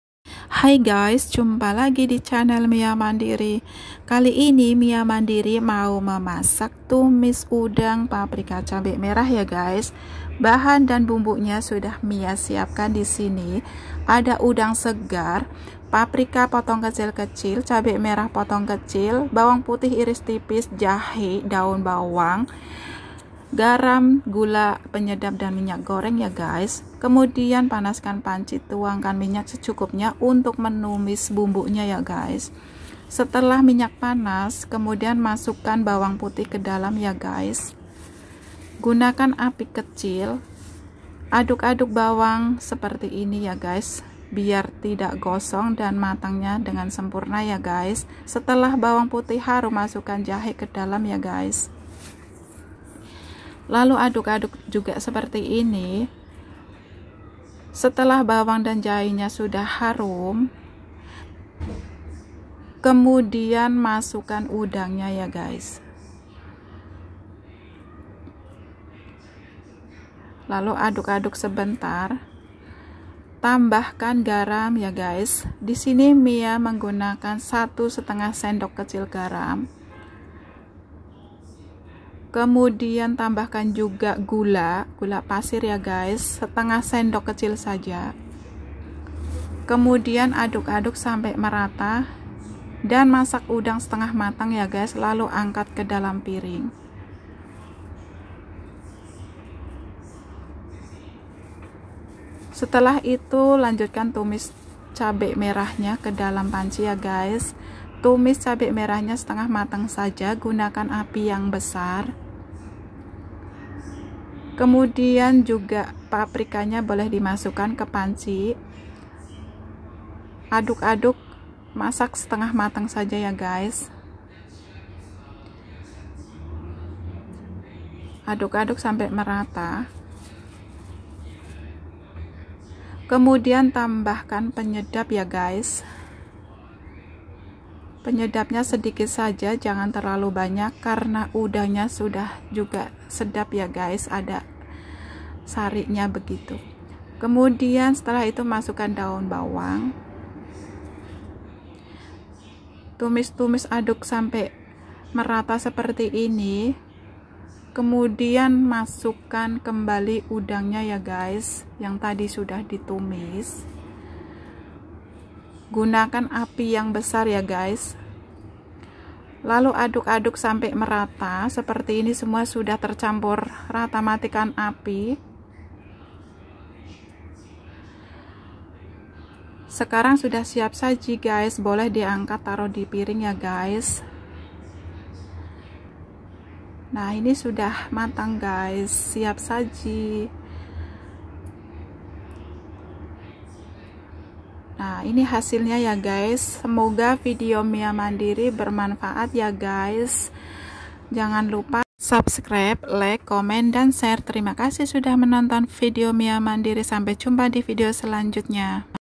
udang segar tumis paprika & cabai merah.👍❤
udang segar tumis cabai merah.

bahan"
udang segar.
paprika.
cabai merah.
bawang putih.
jahe.
daun bawang.
garam.
gula pasir.
penyedap.
minyak goreng.